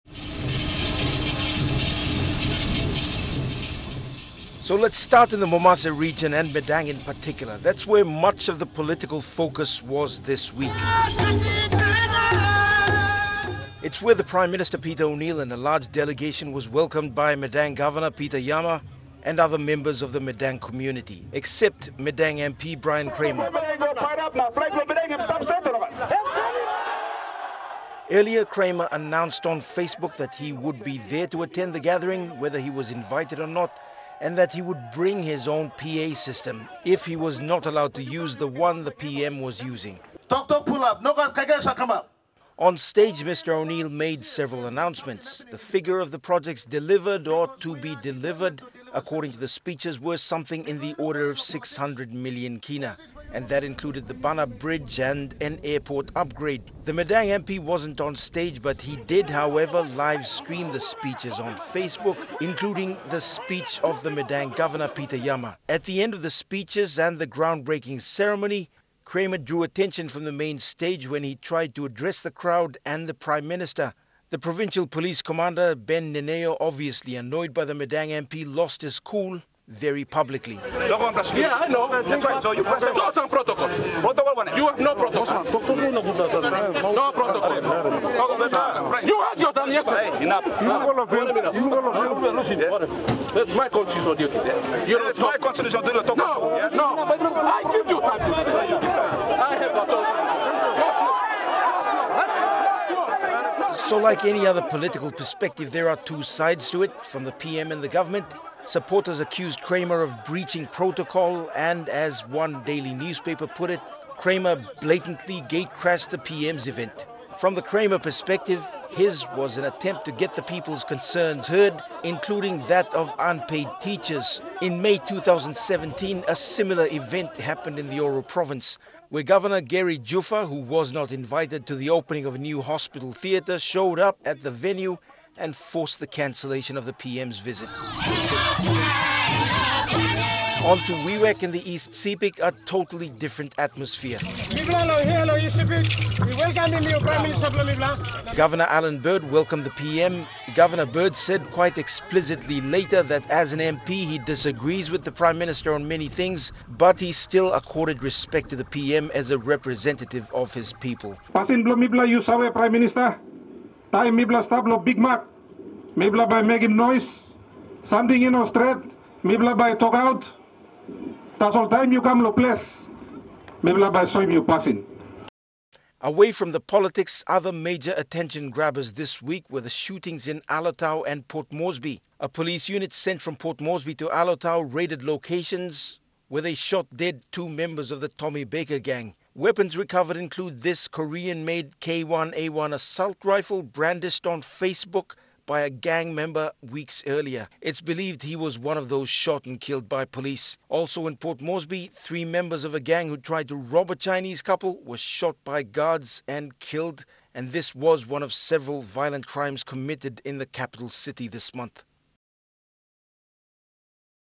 Scott Waide takes a look at some of the events that happened in this WEEK IN REVIEW
This week the Prime Minster and other MPs continued a series of visits to the provinces.

In Madang, tempers flared as the provincial police commander told the Madang MP, Bryan Kramer   not to interfere with the PM’s program due to a breach of protocol.

And again, depending on which side of the house you’re on,  the opposition and government tussle for influence has split audiences – some watching as observers which others activity responding to  political social media trolls.